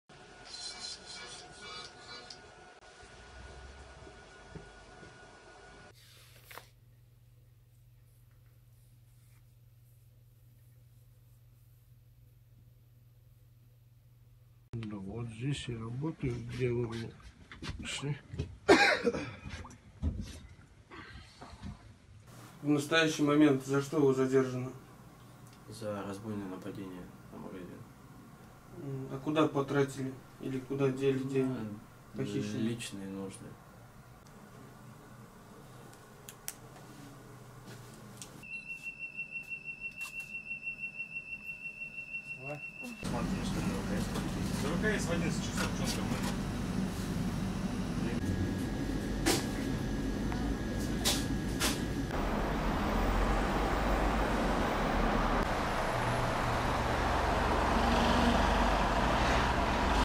В Подмосковье объявились «Бонни и Клайд» с пистолетом
Подмосковные полицейские задержали двух мужчин и беременную женщину, которые с оружием в руках грабили продуктовые магазины Московской области.